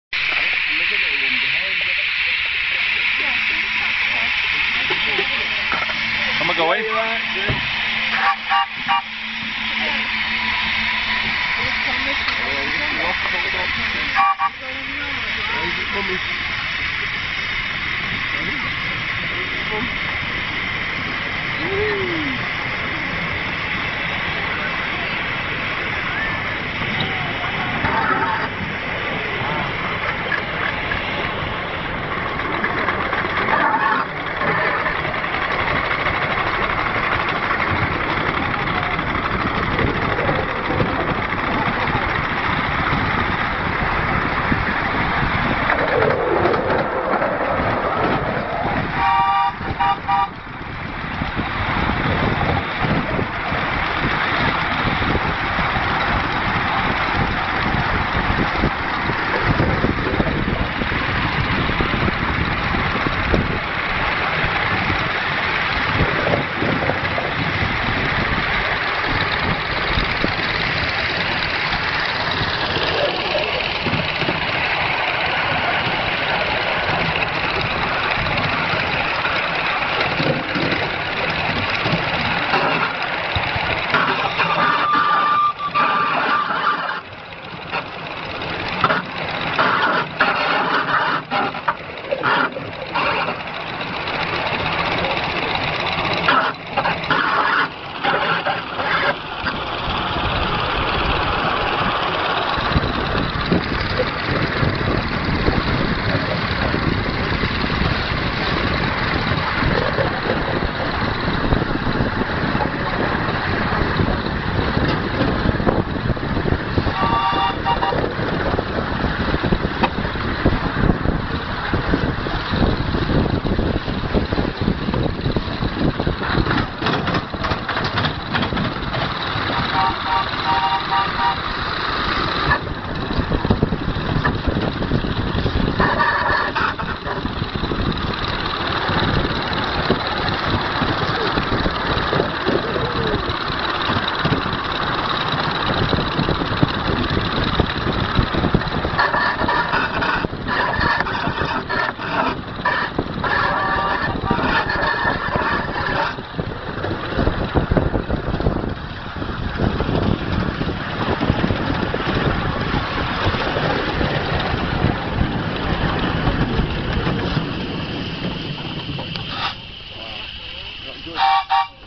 5" live steam loco sweet pea " Sweet Iris"
This is a 5" live steam loco owned by a member at S.A.M.E.S (sale area model engineering society)